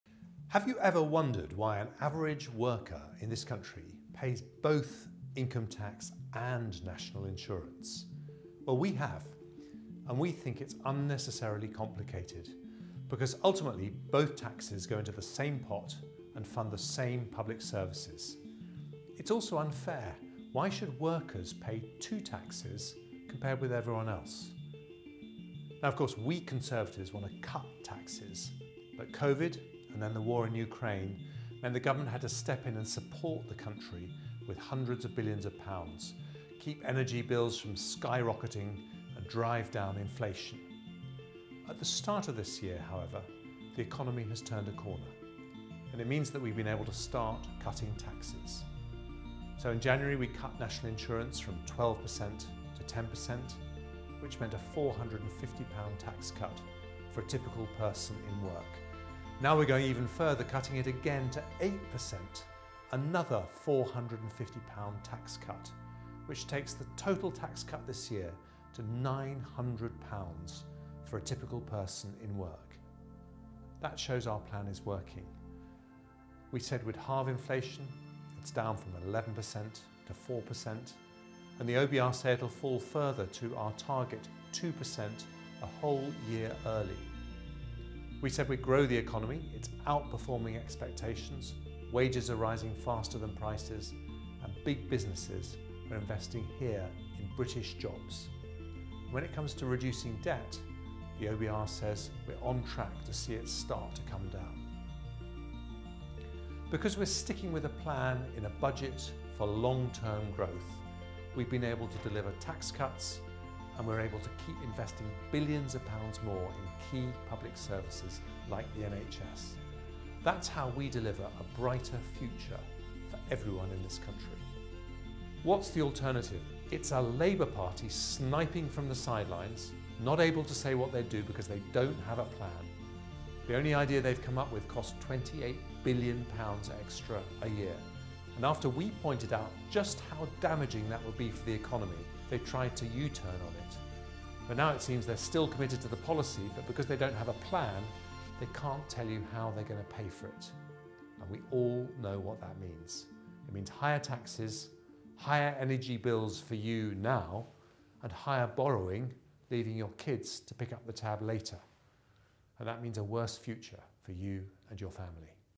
Cutting your Taxes | Chancellor Jeremy Hunt
Chancellor of the Exchequer Jeremy Hunt outlines the Conservatives plan for the economy, which is delivering you tax cuts. Labour have no plan, preferring to snipe from the sidelines. They want to borrow £28 billion, meaning higher taxes for you today, and higher borrowing for your kids and grandkids in the future.